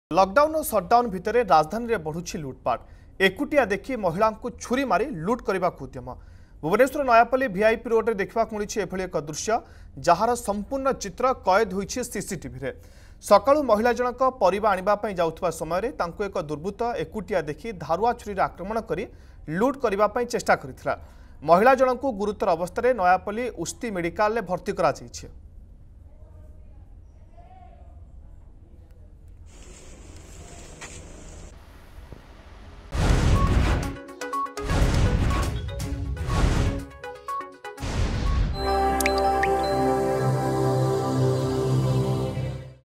Nayapalli VIP Colony Loot Case: ଏକୁଟିଆ ଦେଖି ମହିଳାଙ୍କୁ ଛୁରୀ ମାରି ଲୁଟ କରିବାକୁ ଉଦ୍ୟମ
The crime rate in Bhubaneswar is rapidly increasing during the Covid lockdown. Earlier this morning, a miscreant attacked a woman with a knife.
ଲକଡାଉନ୍ ଓ ସଟଡାଉନ ଭିତରେ ରାଜଧାନୀରେ ବଢୁଛି ଲୁଟପାଟ୍ ।  ଭୁବନେଶ୍ବରର ନୟାପଲ୍ଲୀ ଭିଆଇପି ରୋଡ଼ରେ ଦେଖିବାକୁ ମିଳିଛି ଏପରି ଦୃଶ୍ୟ